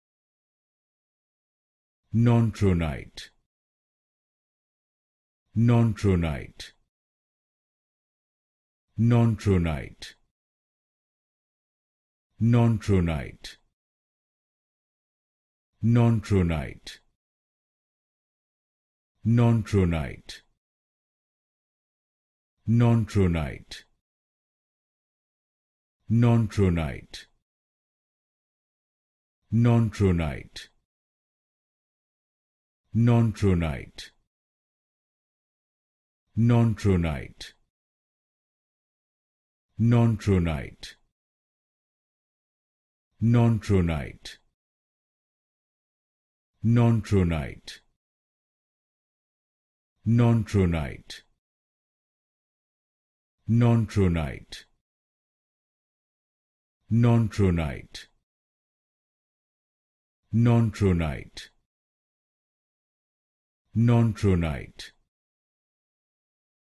Pronunciation of the word(s) "Nontronite".
Pronunciation of the word(s) "Nontronite". Channel providing free audio/video pronunciation tutorials in English and many other languages. The videos cover a diverse range of subjects with a specific focus on technical words that can be extremely challenging to pronounce correctly.